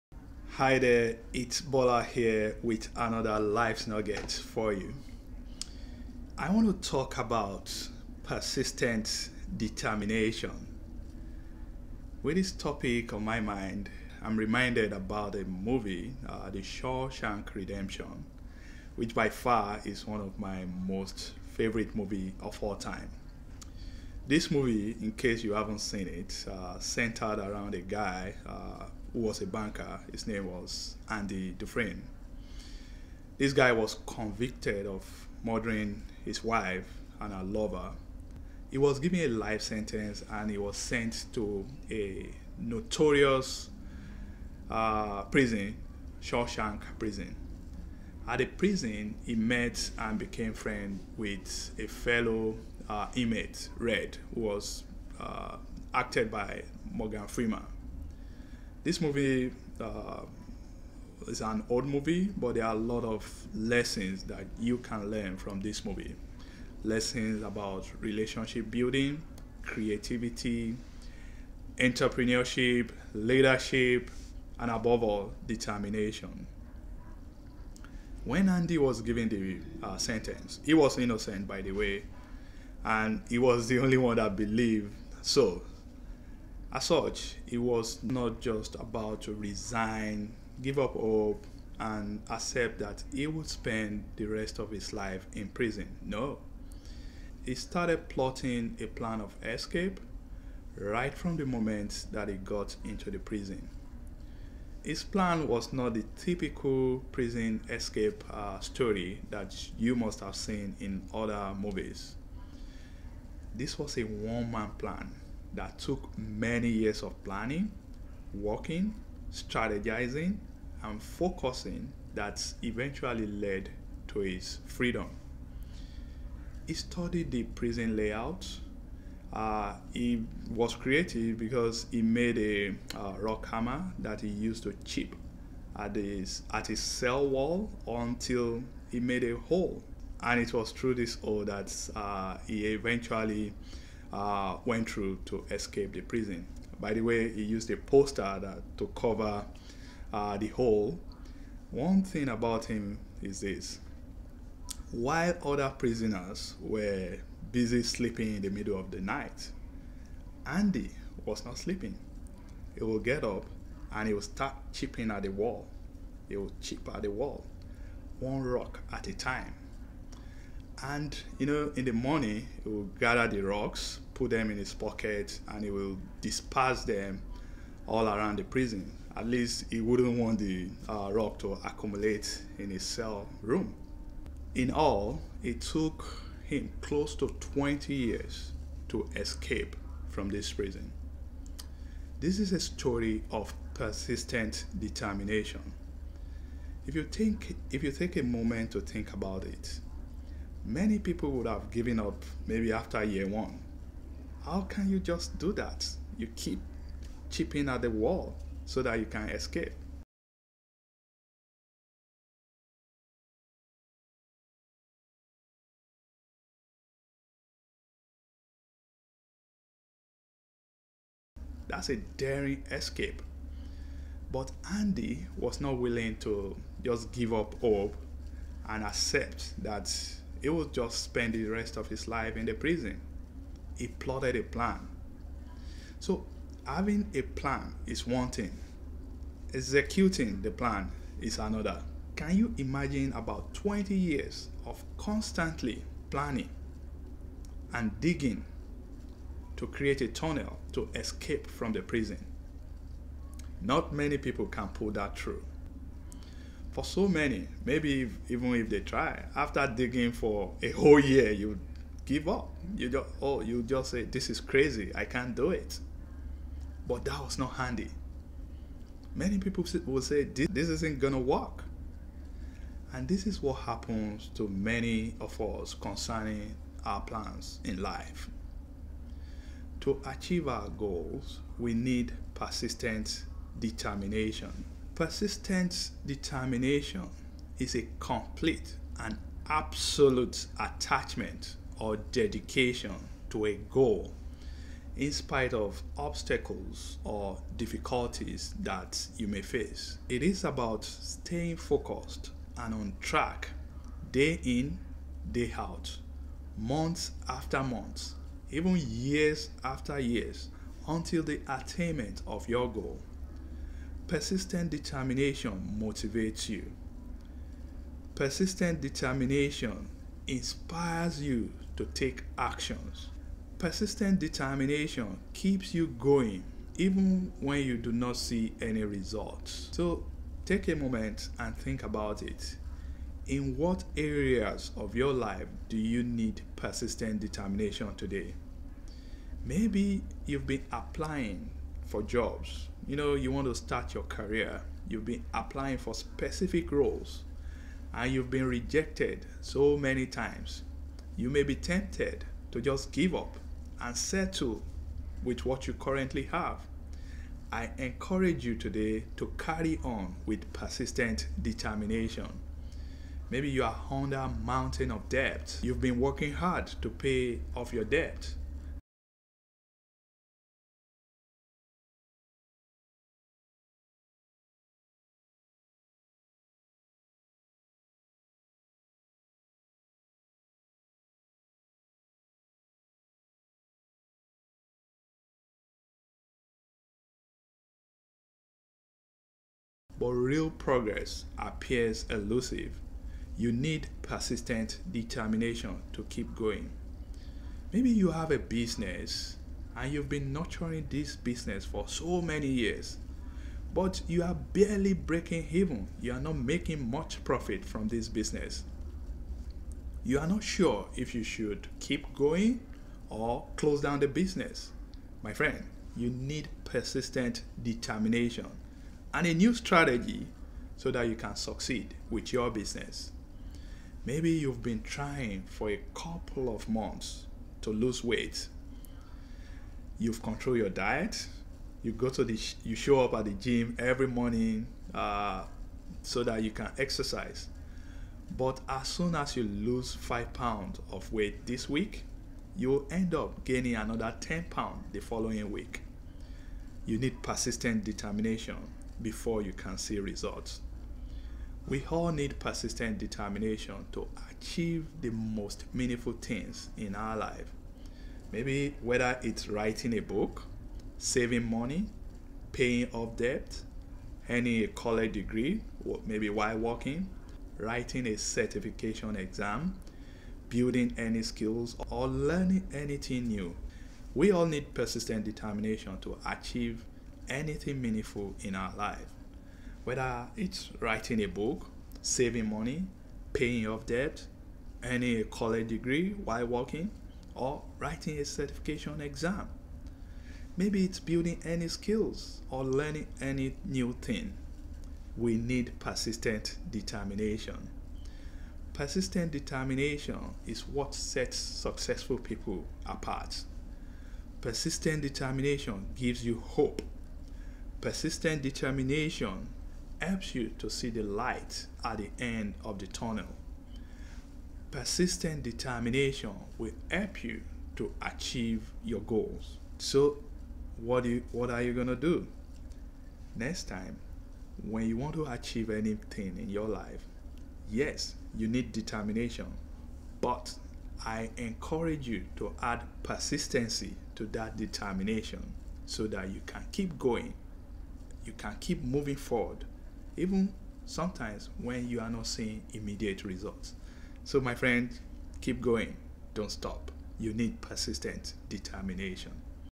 Persistent Determination
Persistent Determination is a complete and absolute dedication to a goal in spite of obstacles or difficulties.

It is about staying focused and on track, day in day out, months after months, even years after years, until the attainment of the goal.

Persistent Determination motivates you.
Persistent Determination inspires you to take action.
Persistent Determination keeps you going, even when you do not see any results.